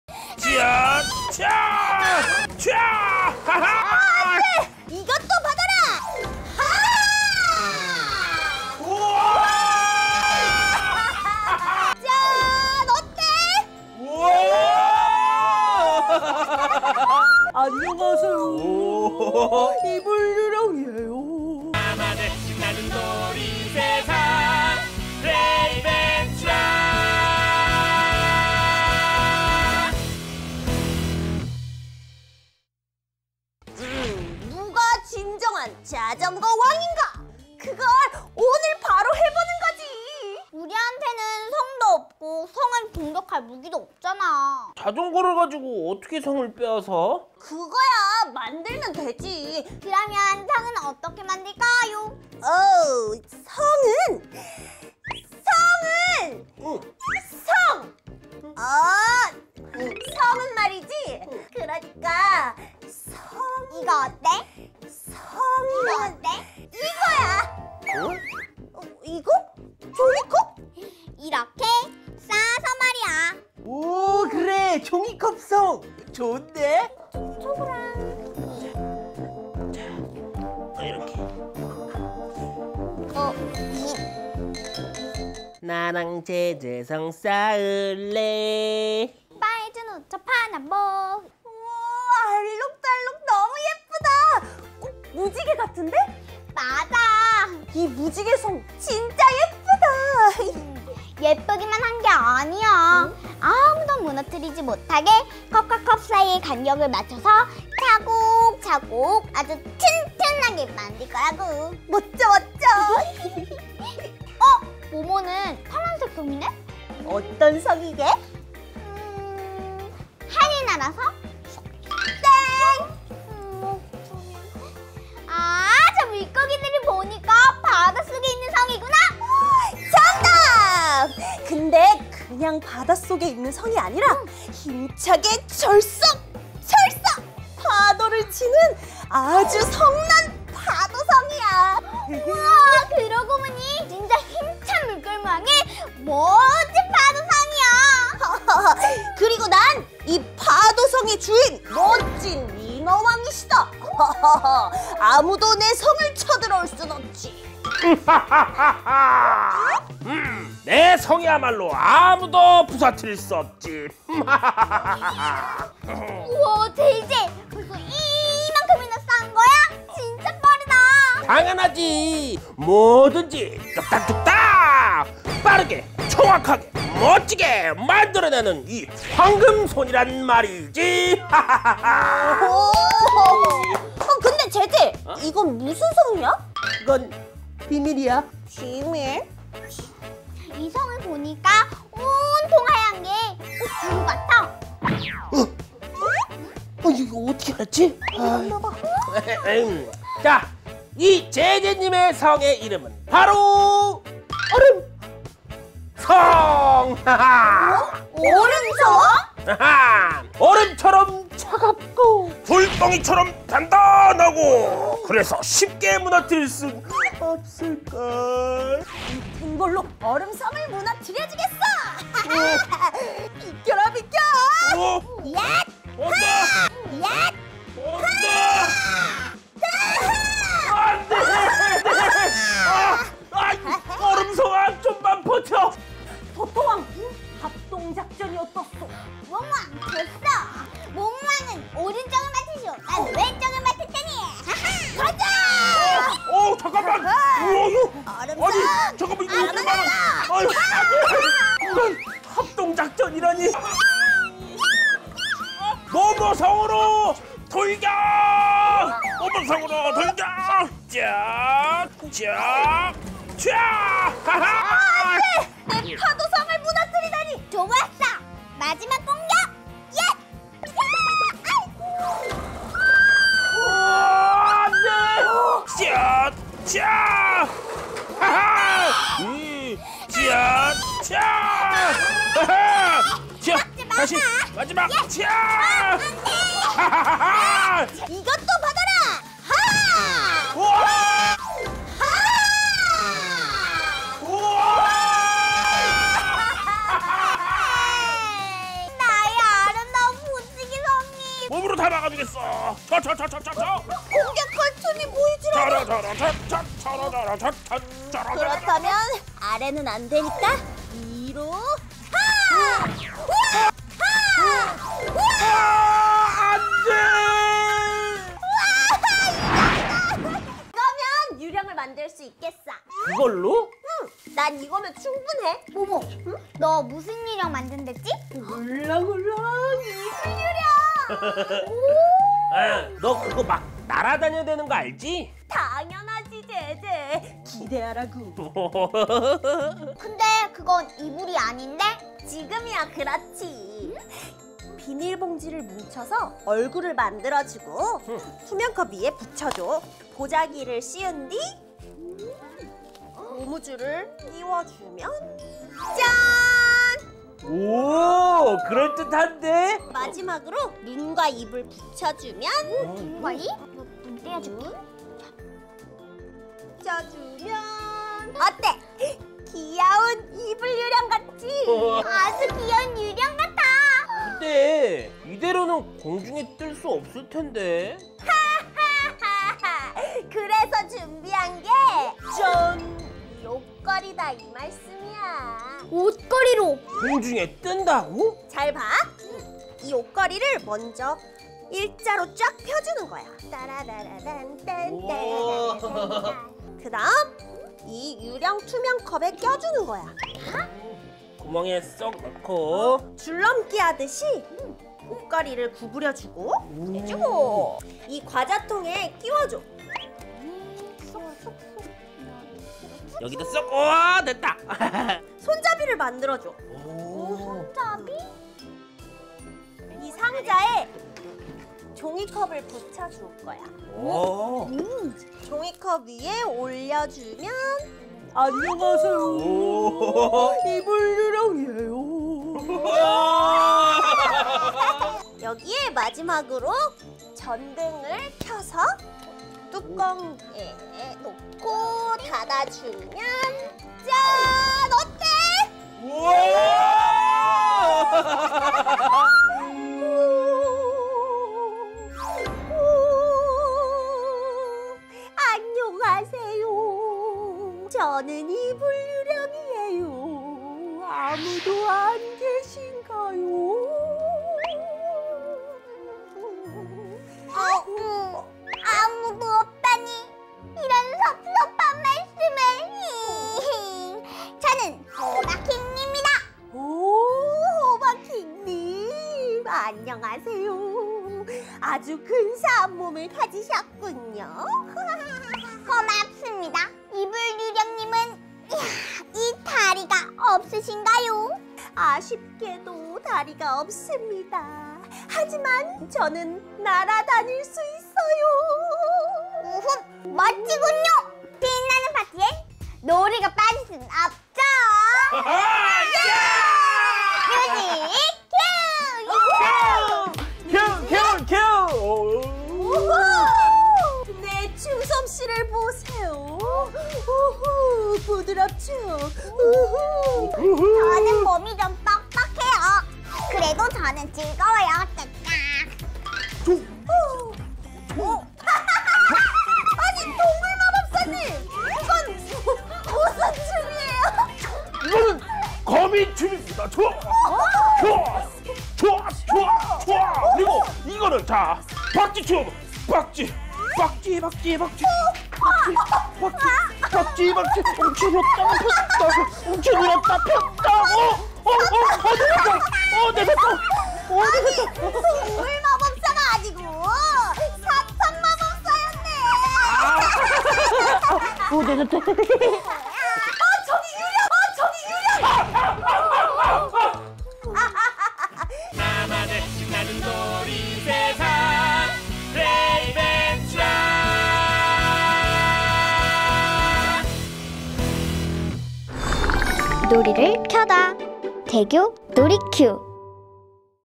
[벤츄라 놀이사전] 🥤기상천외 종이컵 놀이 BEST 2! 🥤
재미있는 놀이를 모았다! 벤츄라 놀이사전 🥤일회용 컵 놀이🥤 편!
종이컵으로 어떤 걸 만들 수 있을까요?
모두 함께 집에서 컵으로 성도 쌓고 유령도 만들며 재미있게 놀아봐요!

▶플레이벤츄라 전체 에피소드를 보고 싶다면?